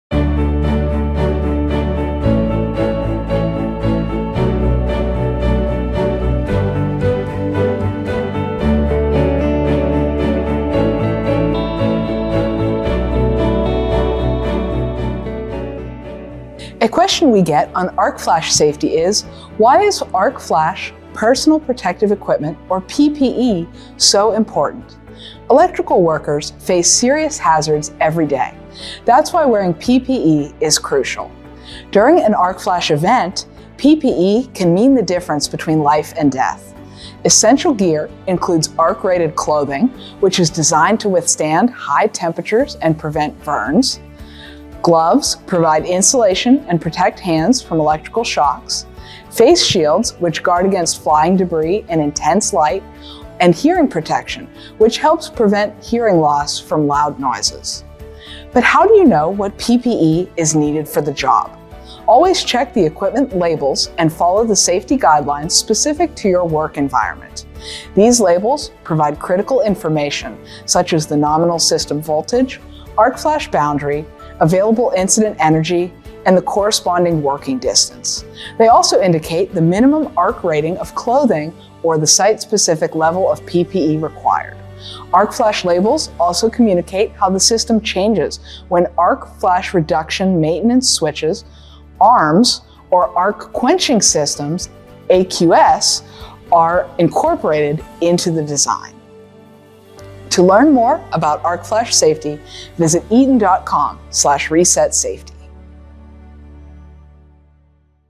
Ask the Expert: Why is arc flash PPE so important? | Eaton
In this video, we delve into the critical importance of arc flash personal protective equipment (PPE) for electrical workers. Arc flash incidents pose serious hazards, and wearing the right PPE can mean the difference between life and death. An Eaton Application Engineer explains that electrical workers face serious hazards every day, making PPE crucial for their safety. The video showcases a montage of different types of PPE, including arc-rated clothing, gloves, face shields, and hearing protection. Each piece of equipment plays a vital role in protecting workers during an arc flash event. Join us as we explore the essential gear that keeps electrical workers safe on the job and learn more about arc flash and arc flash mitigation